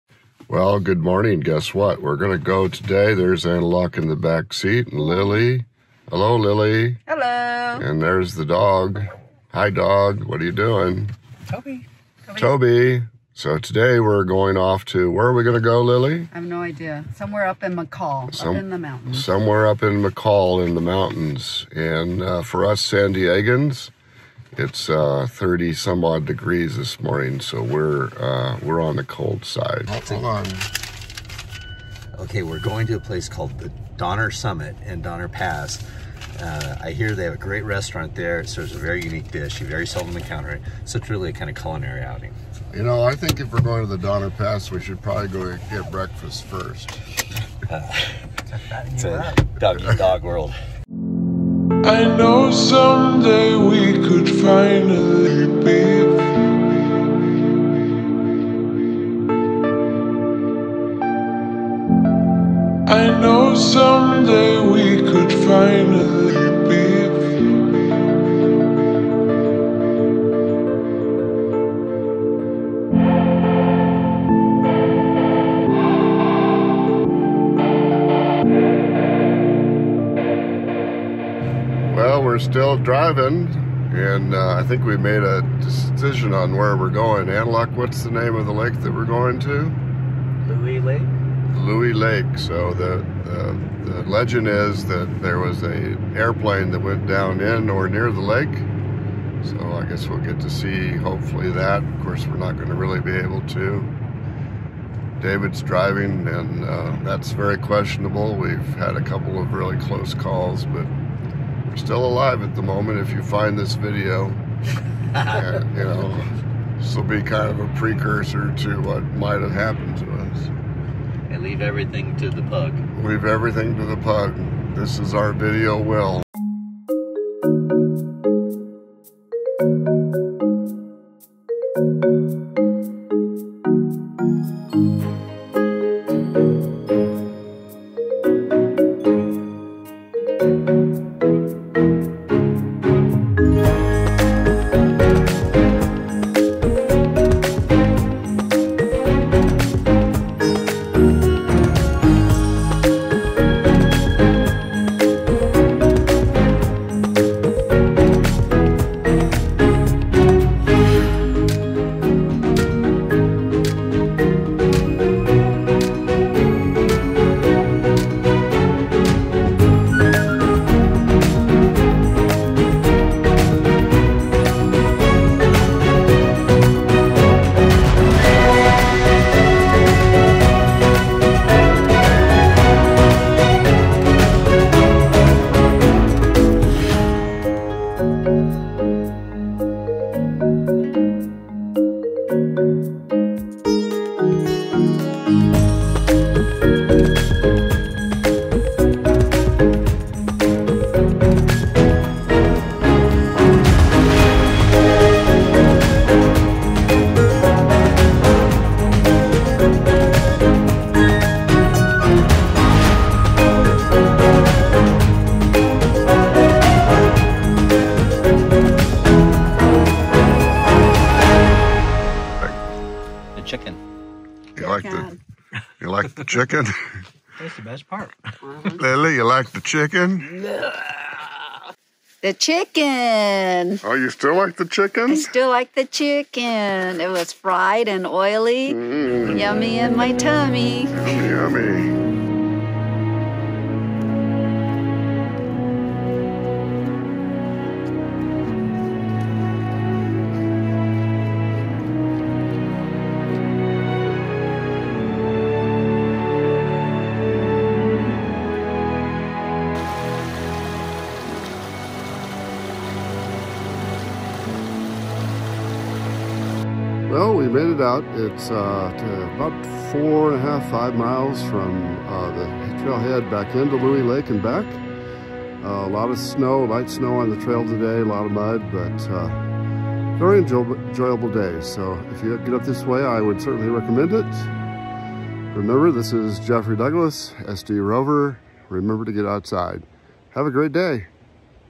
Louie Lake Hike in Idaho
A recent hike to Louie Lake near McCall Idaho.  Beautiful little lake and great hike.